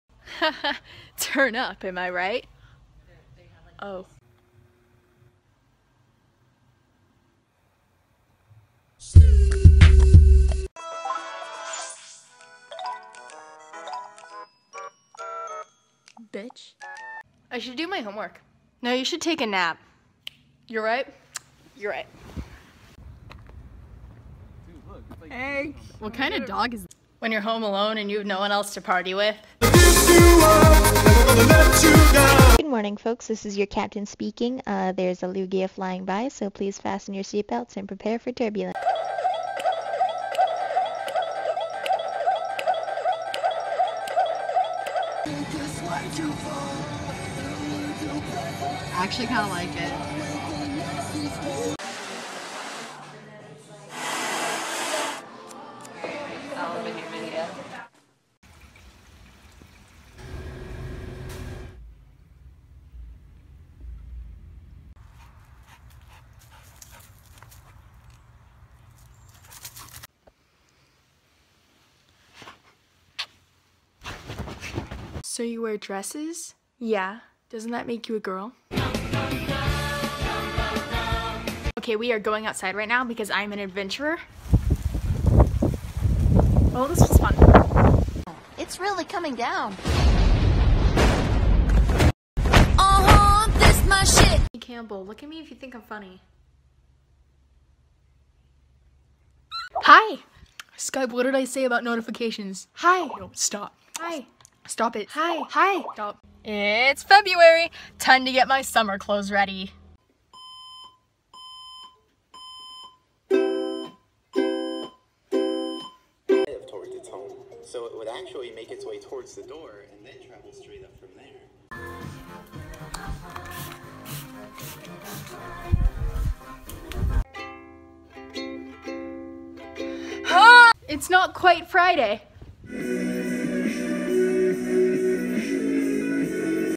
I FOUND MY OLD VINES AND THEY"RE PRETTY BAD
or wow, i used to be funny

edit: i'm nonbinary and use they/them pronouns for people asking!